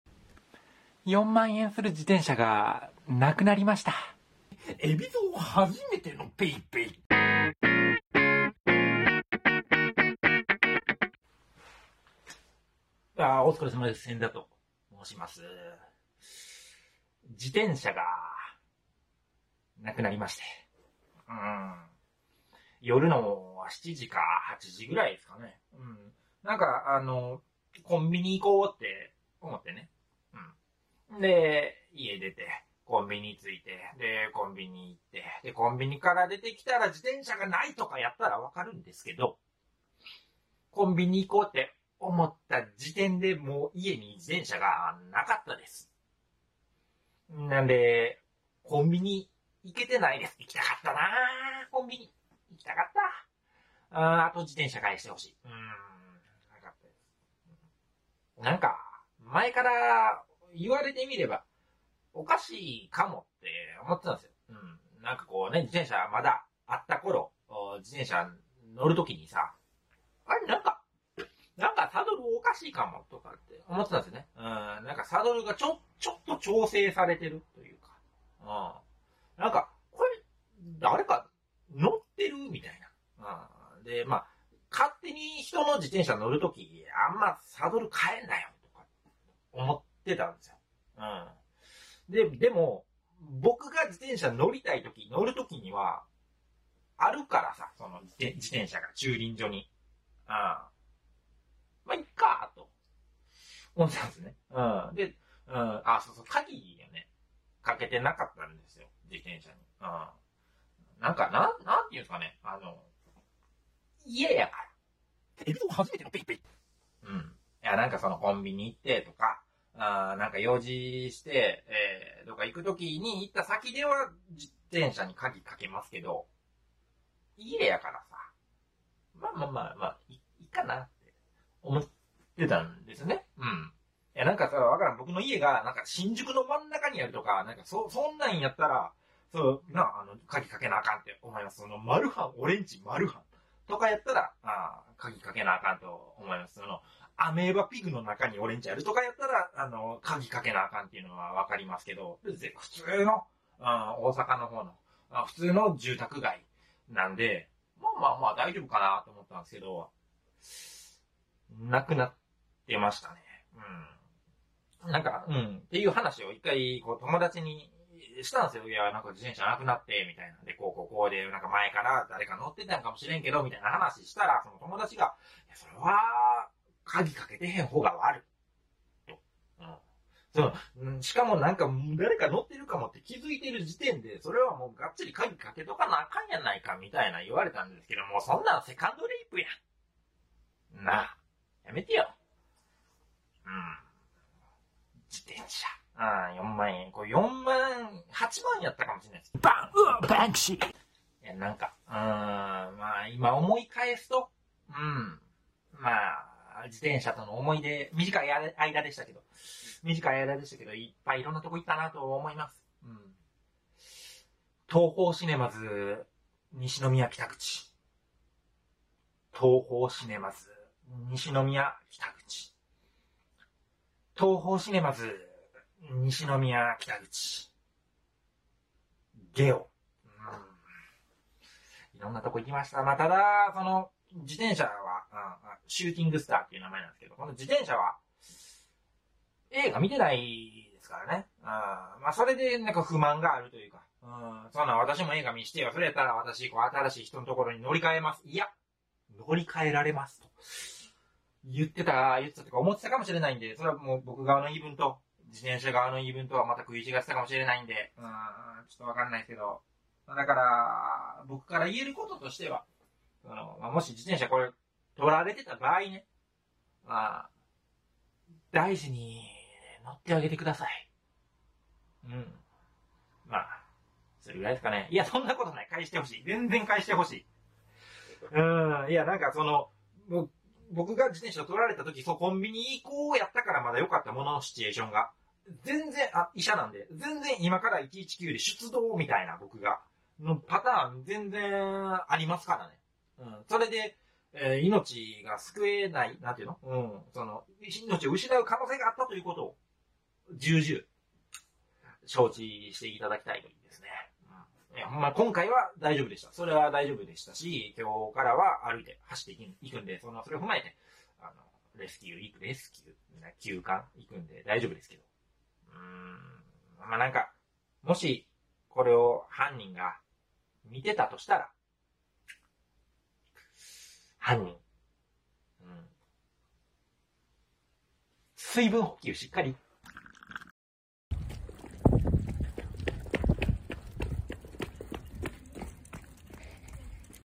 チャリ盗まれたけど、逆にあげます【4万円】
粗品に貰った10万で買い直したらいいやんって言われたけど、なんで俺チャリ盗ったやつに奢らなあかんねん。百歩譲って奢るとして、ほんならチャリ盗った奴もギャグせなおかしいやろ。